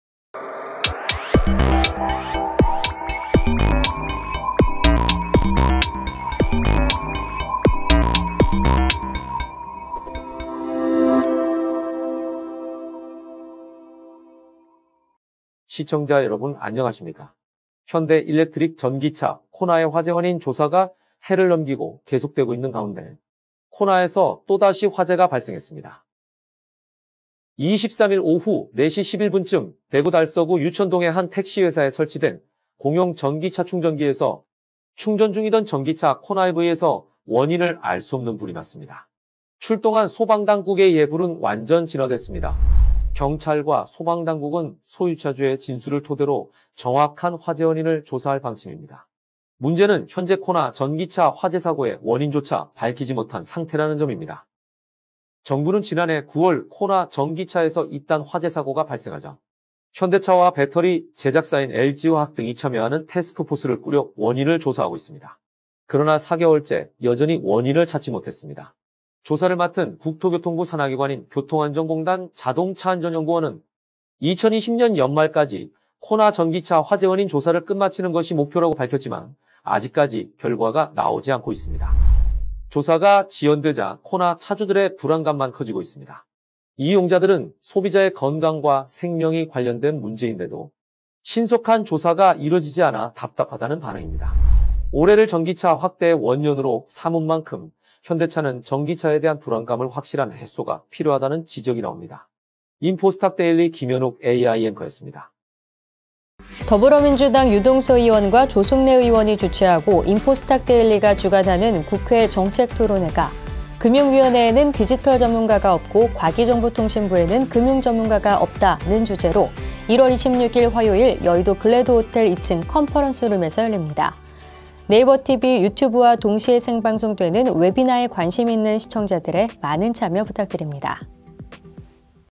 [백브리핑AI] 현대차 '코나 전기차' 또 화재..왜?
코나 전기차, 또 화재 발생

대구 공용충전기에서 원인 불명의 화재

전기차 화재의 원인 파악 못 해

현대차+LG화학 TF 4개월째 원인 못 찾아

자동차안전연구원, 2020년 12월 종료 시한 넘겨

소비자, 조사 지연에 불안감 호소